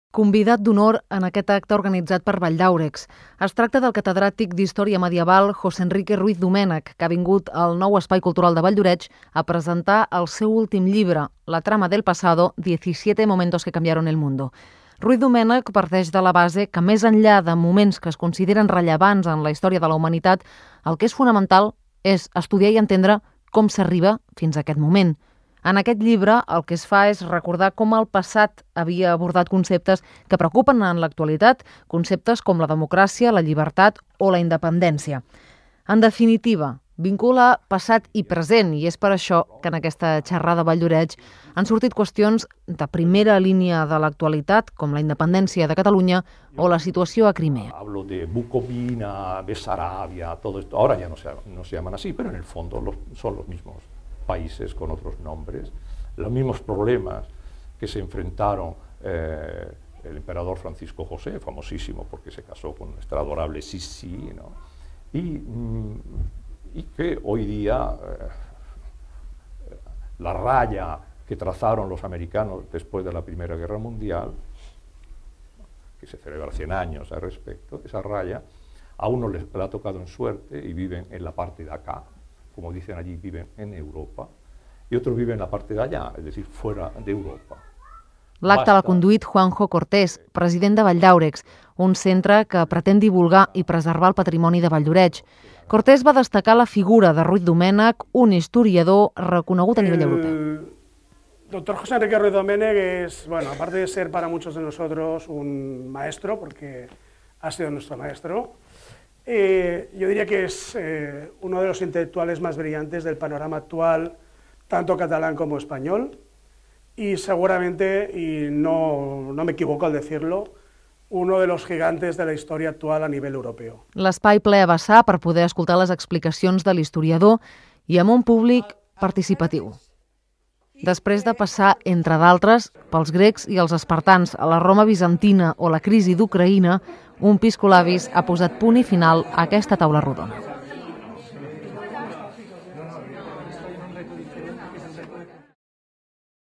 Presentació del Llibre "La trama del Pasado" a Valldoreix
El passat divendres Valldaurex va organitzar una xerrada col·loqui amb l'autor del Llibre"La trama del pasado" José Enrique Ruiz-Domenech: